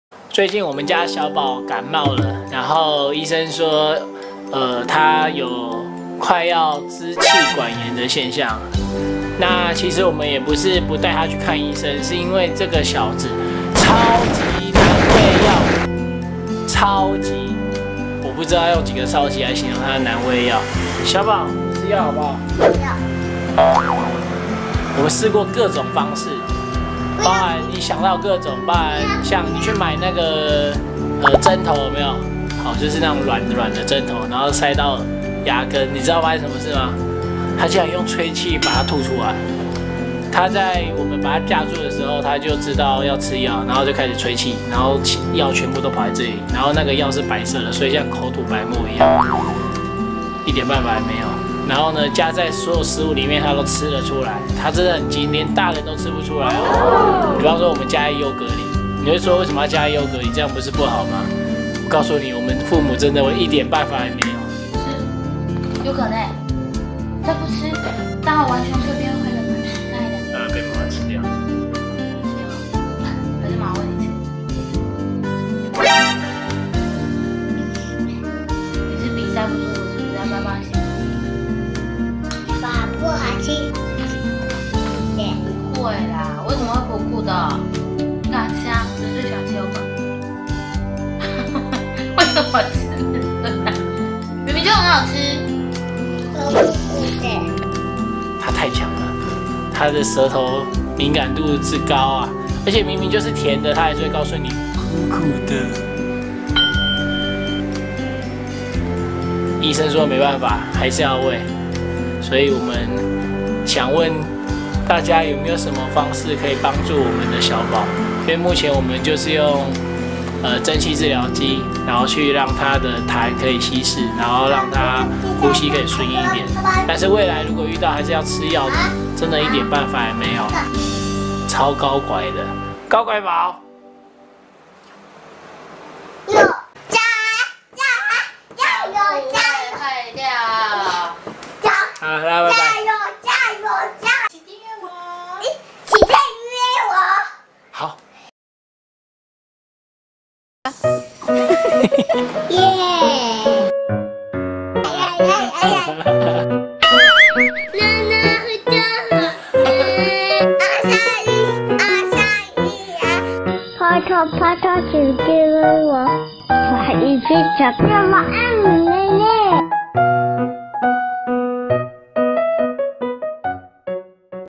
不吃藥的小孩｜Do not take medicine【 love TV小寶愛你笑】
家家有本難念的經，強力徵求各種成功的餵藥方法！！

嘗試過的方法
好好溝通直接餵→拒絕
用灌的→嗆到，嚇死我們。
餵藥器→利用吹氣的方式吐出來
手指沾藥粉塗口腔內側→用濃稠的口水吐出來，弄得滿身。
加在奶裡→睡著才餵的進去，但這樣會降低藥效
加在優格→被吃出來，拒絕再吃。